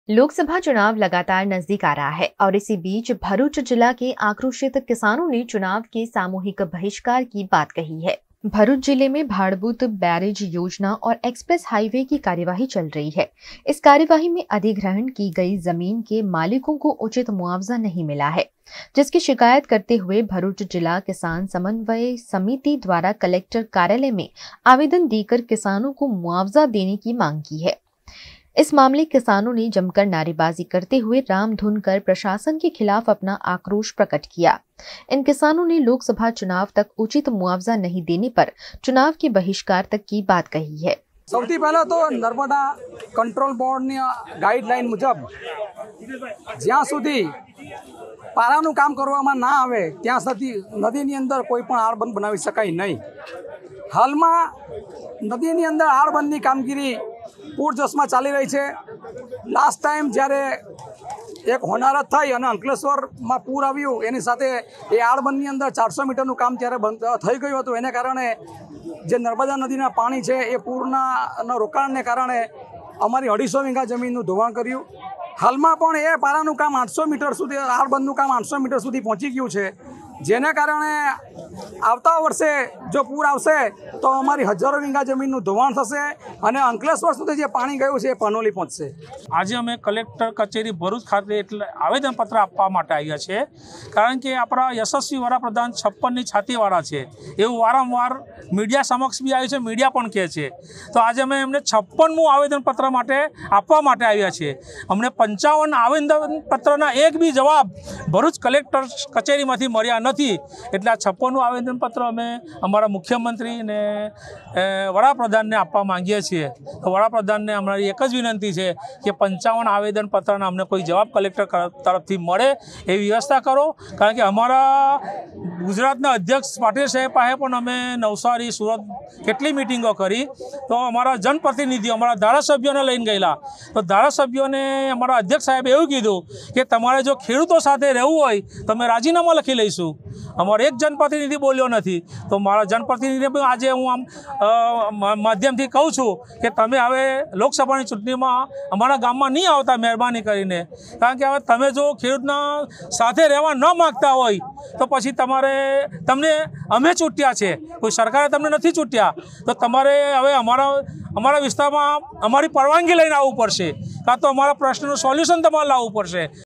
भरूच जिले के किसान करेंगे लोकसभा चुनाव का बहिष्कार | VNM
लोकसभा चुनाव लगातार नजदीक आ रहा है और इसी बीच भरूच जिला के आक्रोशित किसानों ने चुनाव के सामूहिक बहिष्कार की बात कही है।